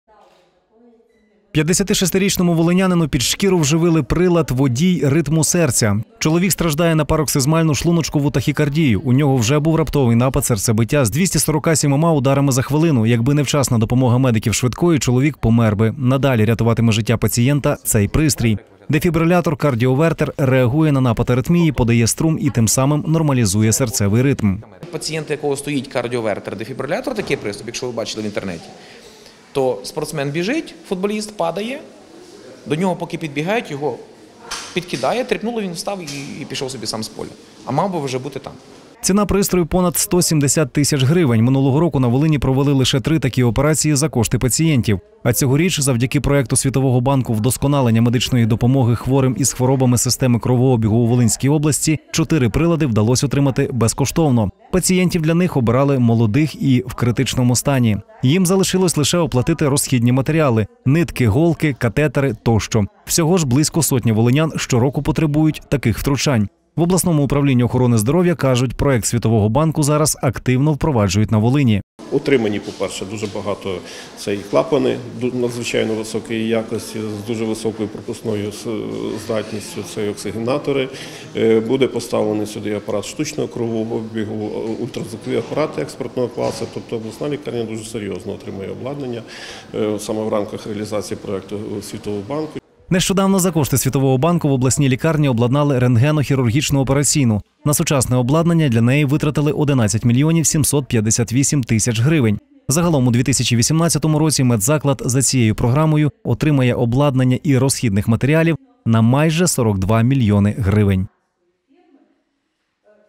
Світовий банк фінансує медицину на Волині
56-річному волинянину під шкіру вживили прилад-водій ритму серця. Чоловік страждає на пароксизмальну шлуночкову тахікардію. У нього вже був раптовий напад серцебиття з 247 ударами за хвилину. Якби не вчасна допомога медиків "швидкої", чоловік помер би. Надалі рятуватиме життя пацієнта цей пристрій. Дефібрилятор-кардіовертер реагує на напад аритмії, подає струм і тим самим нормалізує серцевий ритм.